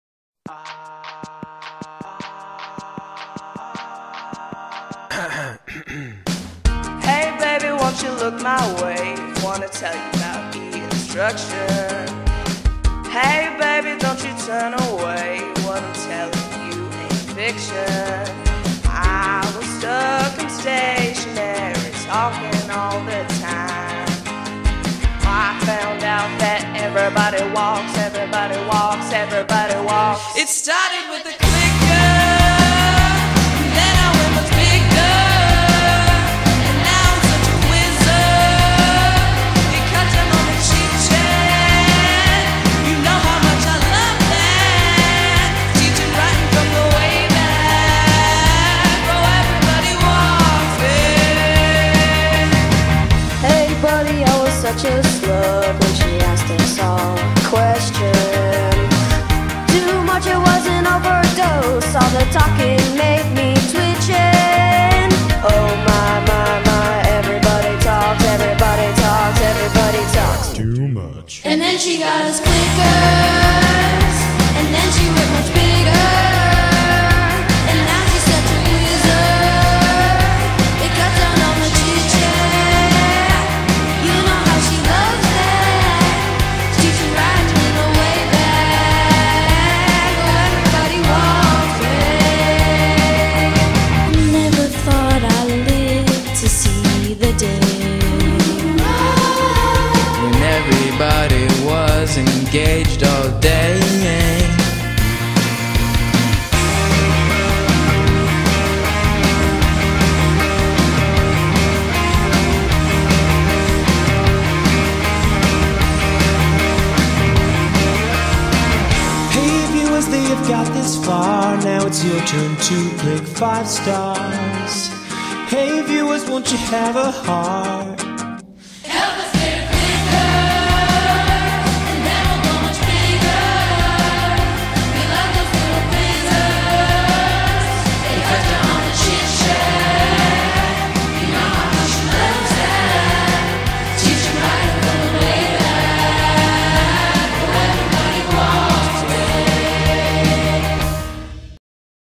It Started With a CLICKER
Weimar Hills Charter School's 8th Grade music video project under the direction of Mr. Oates. This video was submitted as part of a competition to win a classroom tech makeover from einstruction. Vote for us STARTING NOV 1. and passs it along